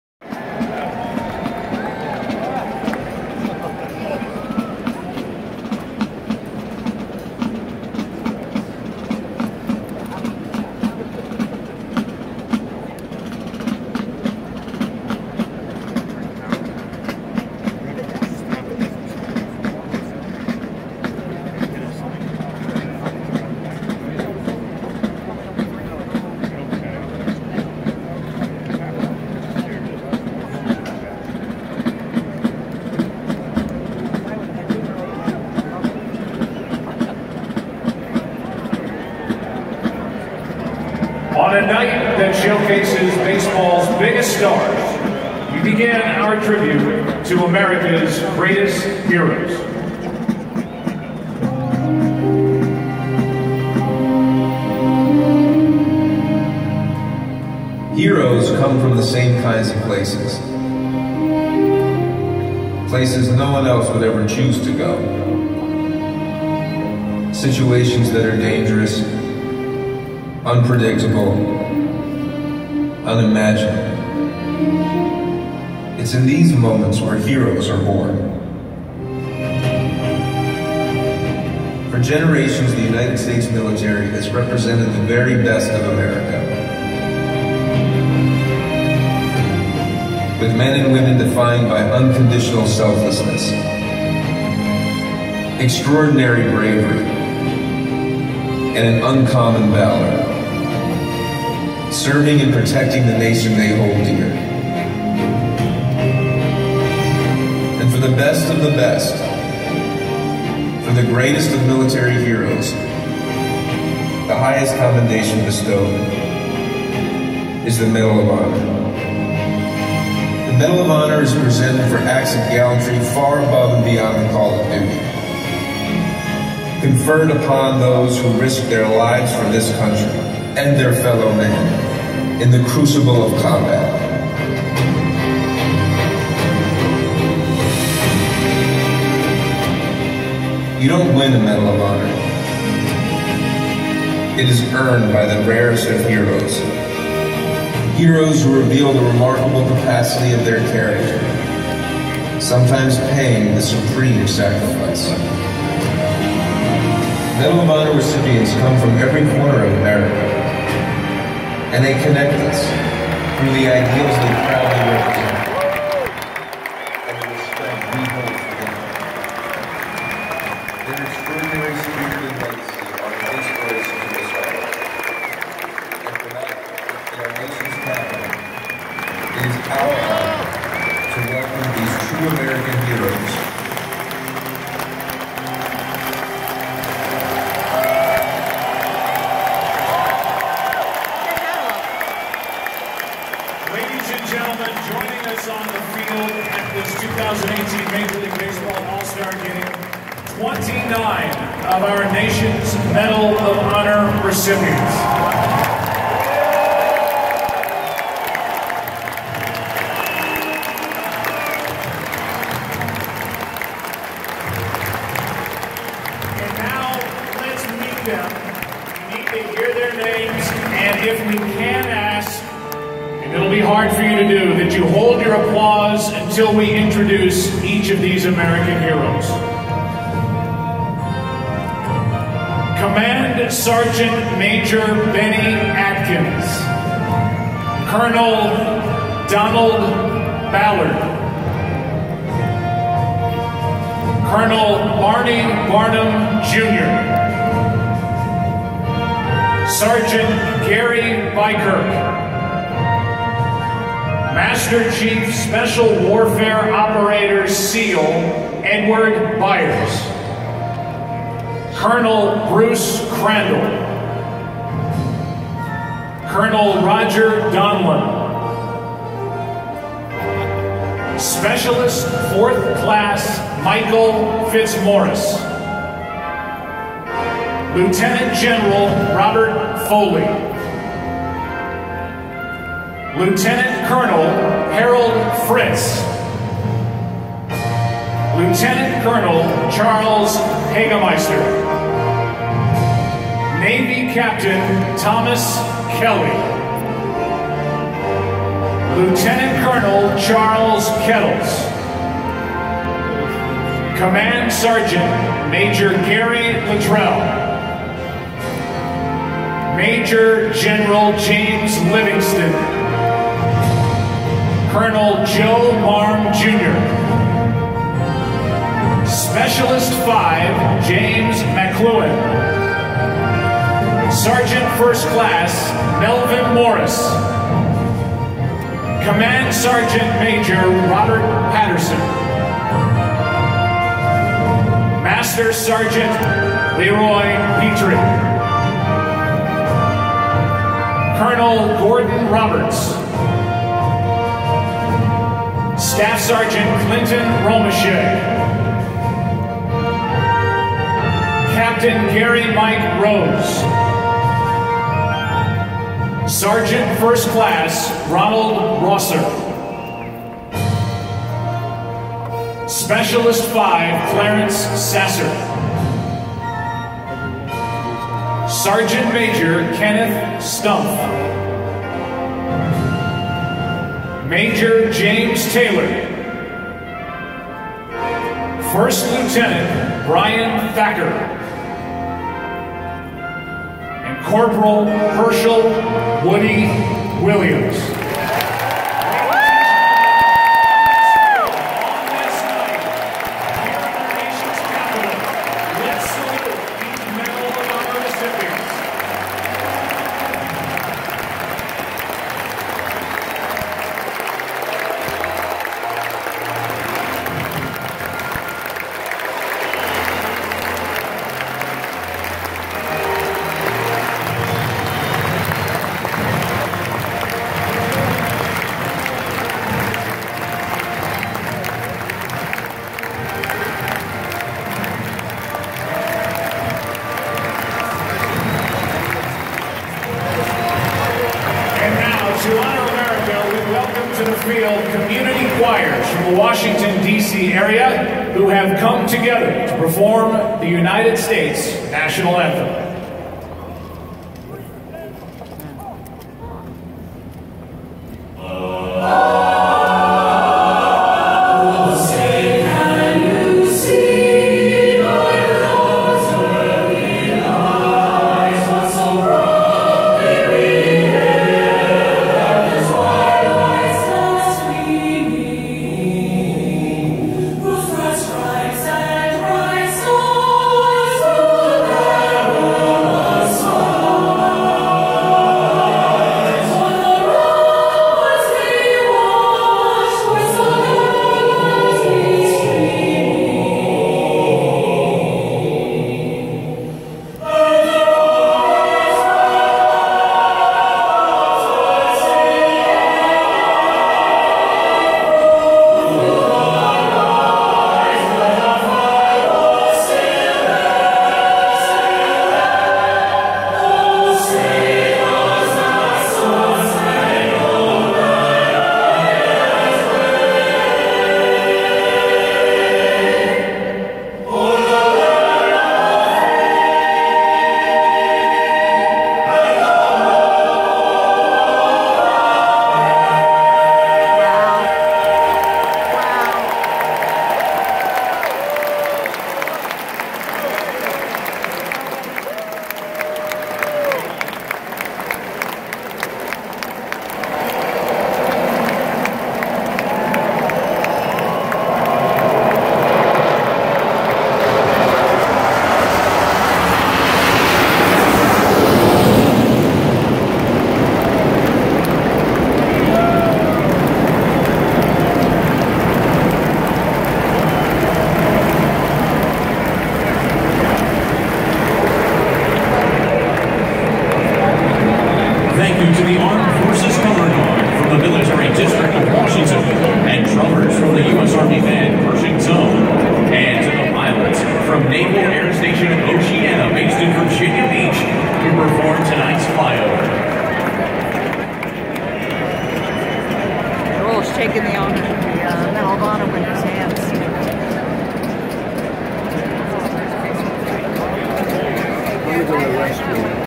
Opening Ceremonies at the 2018 All Star Game
Nationals Park, Washington DC, July 17, 2018